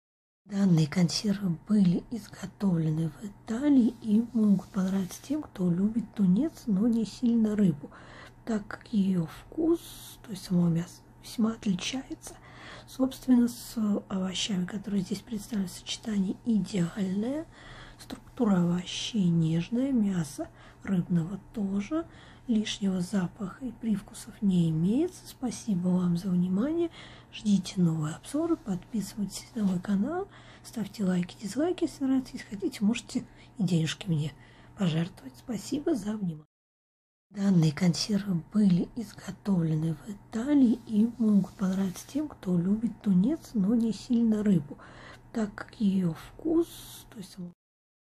Салат из тунца с кукурузой RIO mare обзор/Tuna salad with corn RIO mare review
An unusual combination of vegetables and fish, that is, the main product, which does not quite taste like a sea inhabitant. Weight 160 grams, the price per piece is more than 200 Russian rubles. The composition is perfect, the packaging is metal and cardboard. The taste is pleasantly bright, as are the colors: orange, yellow, black, green, pink. The smell is pleasant, and the structure is delicate.

Необычное сочетание овощей и рыбы, то есть основного продукта, который на морского жителя на вкус не совсем похож.
Вес 160 грамм, цена за штуку больше 200 рублей российских. Состав идеален, упаковка это металл и картон.
Вкус приятно яркий, как и цвета: оранжевый, желтый, черный, зеленый, розовый. Запах приятен, а структура нежная получается.